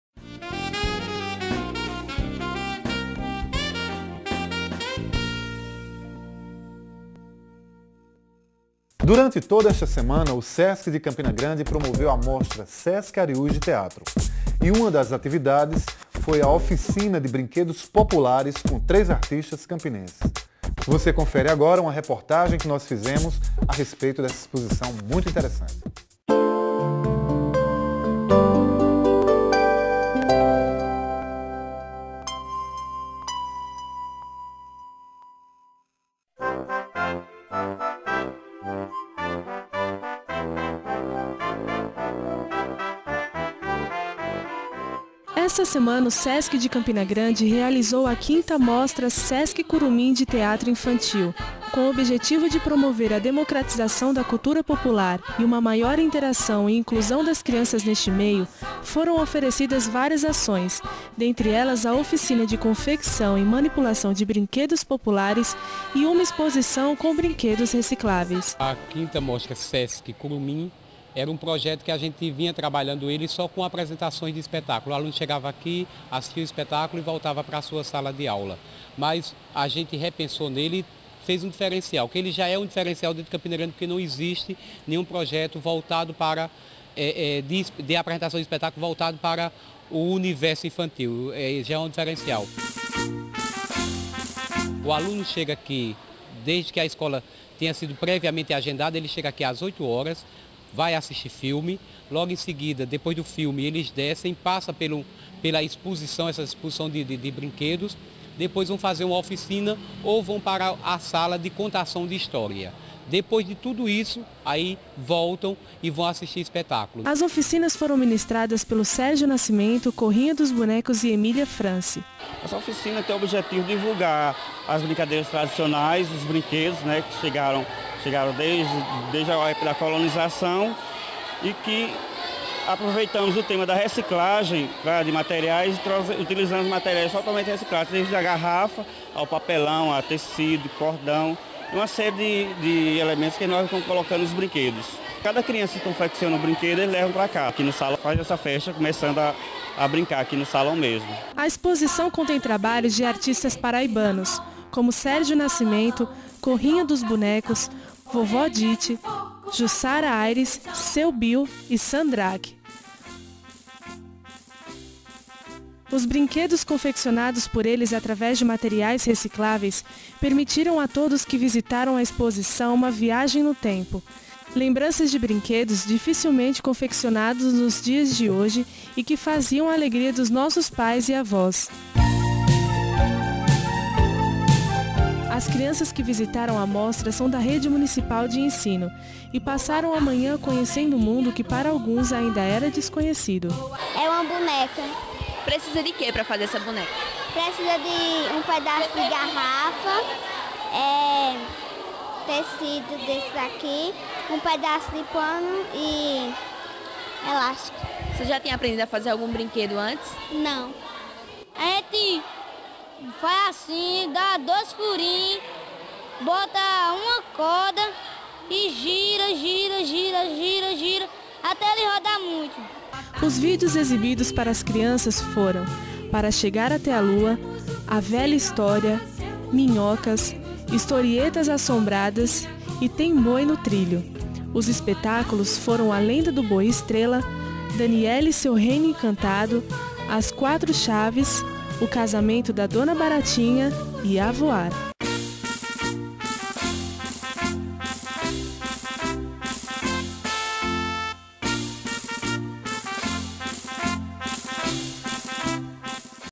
Brinquedos Populares
Matéria sobre a mostra de brinquedos populares promovida pelo SESC de Campina Grande em abril de 2008.